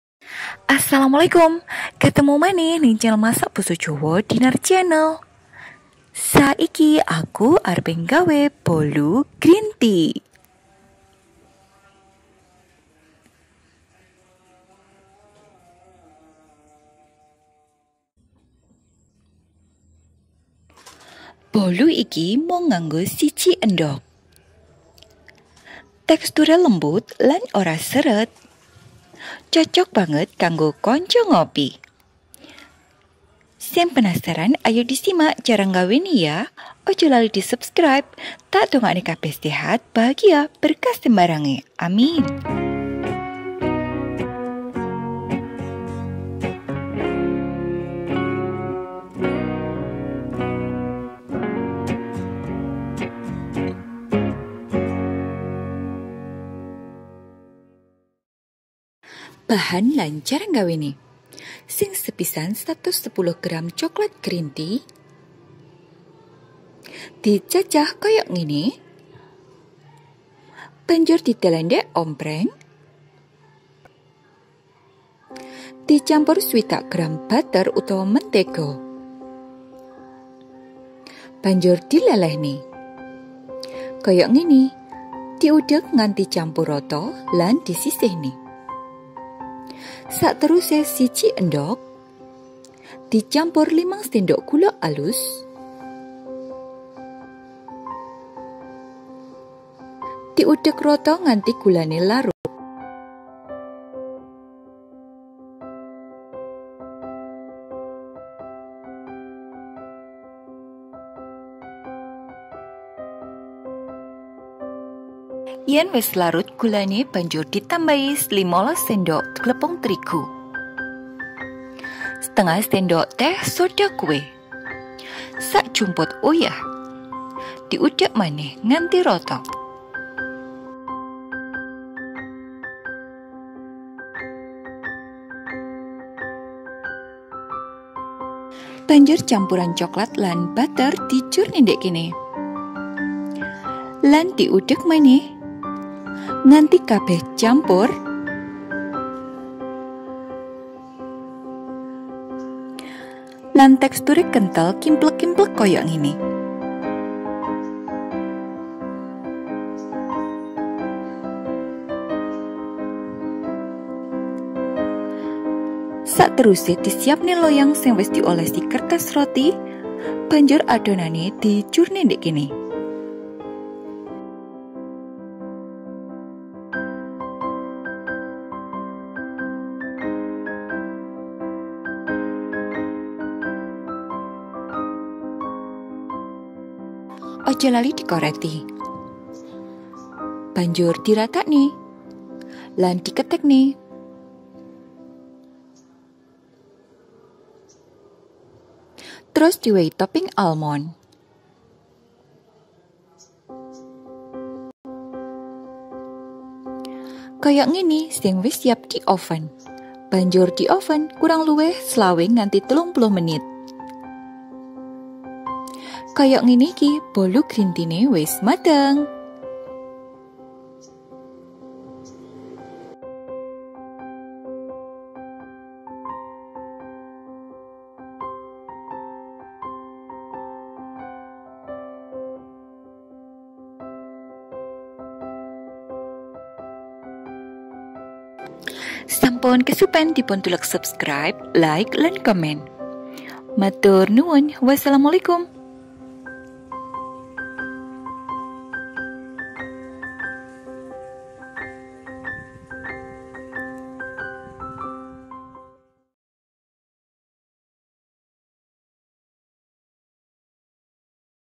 RECIPE #159: Bolu Greentea 1 Telur Tanpa Mixer Oven Tangkring
Assalamualaikum
Apa kabar semua?
Semoga selalu sehat, bahagia, berkah semuanya, aamiin😊🙏
Kali ini saya membuat cemilan praktis teman ngopi disore hari.
Rasanya lembut dan tidak seret.
Selamat mencoba😊

Bolu Greentea

Bahan-bahan:
110 gram coklat batangan rasa greentea
60 gram butter
1 butir telur
5 sdm gula halus
15 sdm tepung terigu
1/2 sdt soda kue
sejumput garam

Topping:
secukupnya kacang almond